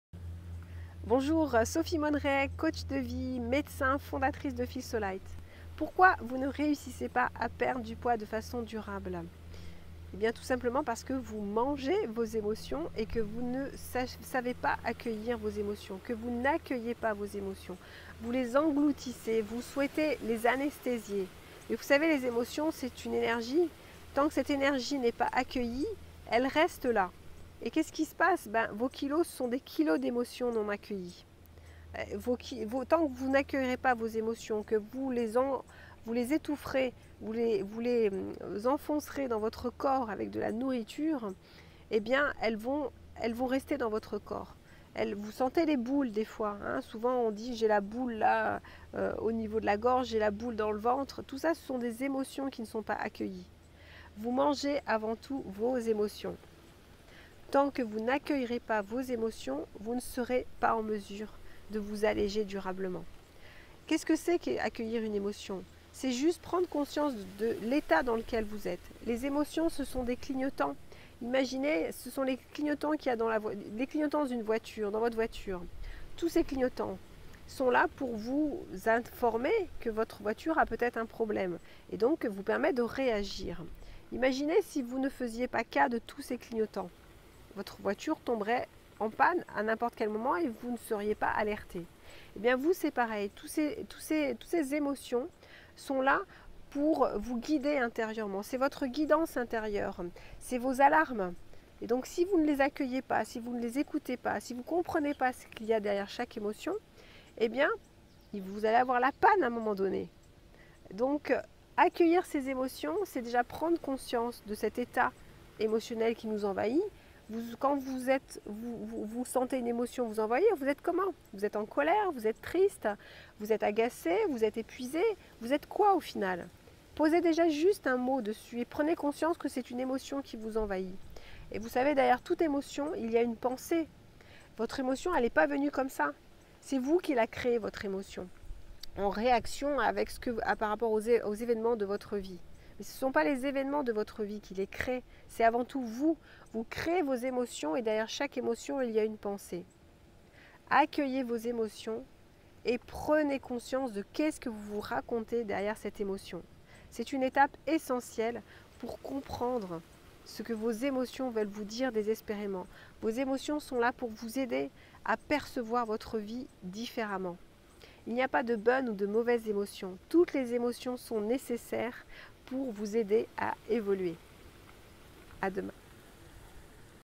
RAISON n°6 : Pourquoi ne réussissez-vous pas à perdre du poids durablement ?
Vous mangez avant tout vos émotions car vous ne savez pas accueillir vos émotions !

Sophie Monneret, coach de vie, médecin, fondatrice de Feel So'light et créatrice du programme en ligne Evolution Minceur